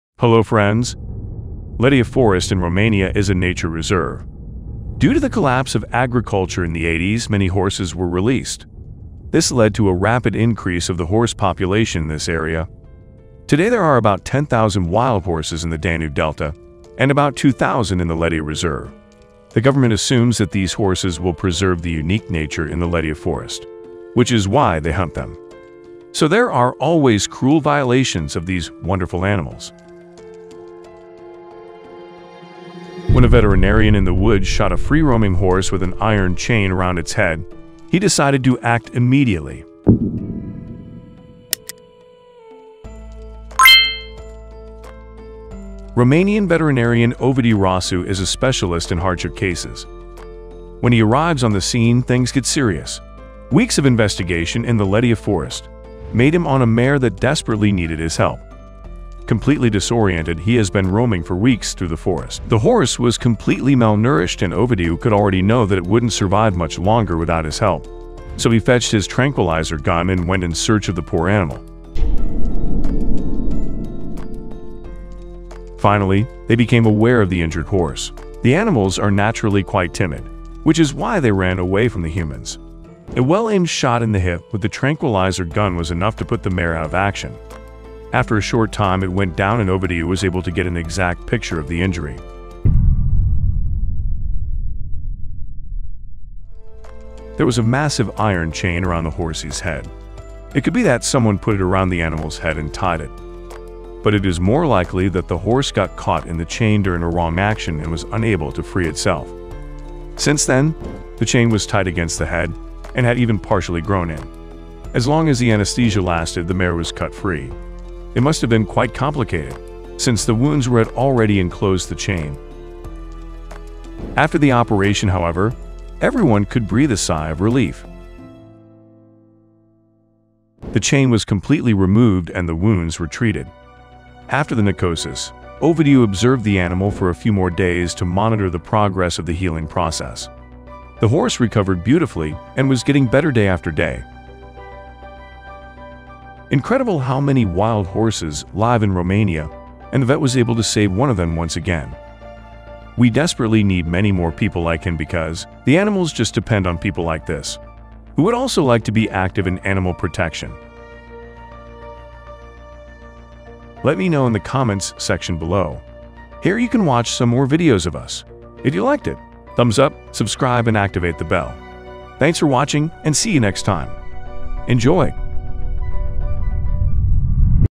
wild horse was saved by this Veterinarian ! horse rescue moment
Hello friends, Letea forest in Romania is a nature reserve. Due to the collapse of agriculture in the 80's, many horses were released. This led to a rapid increase of the horse population in this area.
Today there are about 10,000 wild horses in the Danube Delta and about 2000 in the Letea Reserve. The government assumes that these horses will preserve the unique nature in the Letea forest, which is why they hunt them.
So there are always cruel violations of these wonderful animals.
When a veterinarian in the woods shot a free-roaming horse with an iron chain around its head, he decided to act immediately. Romanian veterinarian Ovidiu Rosu is a specialist in hardship cases.
When he arrives on the scene, things get serious. Weeks of investigation in the Letea forest made him on a mare that desperately needed his help.
Completely disoriented, she had been roaming for weeks through the forest.
She was completely malnourished and Ovidiu could already that she wouldn't survive much longer without his help, so he fetched his tranquilizer gun and went in search of the poor animal.
Finally, they became aware of the injured horse.